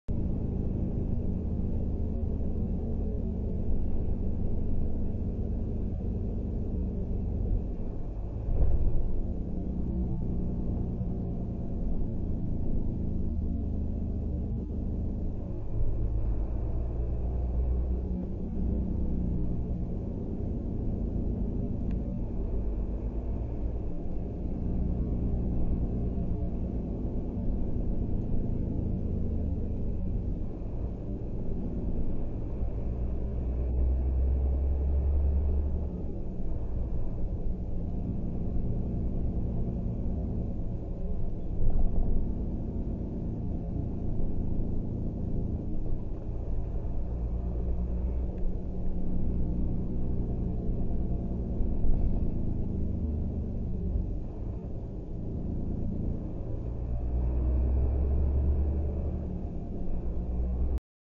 Volvo FM on Grate Ocean Road Near Lorne
Travelling through Grate Ocean road Australia with Volvo FM